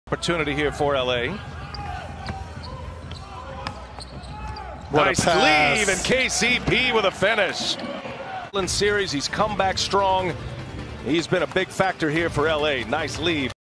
Rondo playoff Skills
Playoffs Rondo shows off his experience, poise and passing skills. Simple pick and slip. Beauty in action.